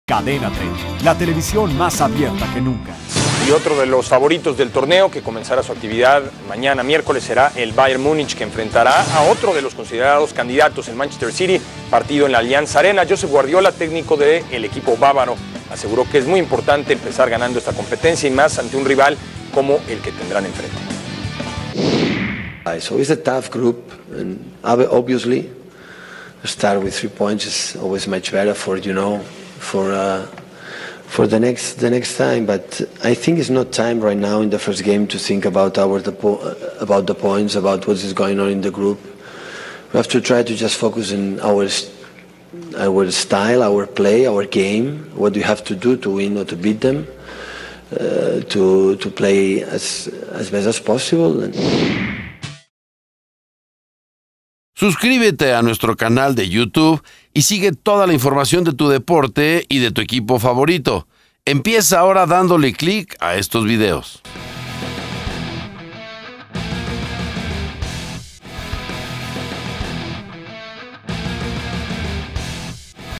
Bayern Múnich vs Manchester City, duelo estelar en la Champions League
Bayern Múnich debutará en la nueva edición de la Champions League ante el Manchester City, su principal rival en un Grupo E que completan Roma y CSKA de Moscú.  17/09/14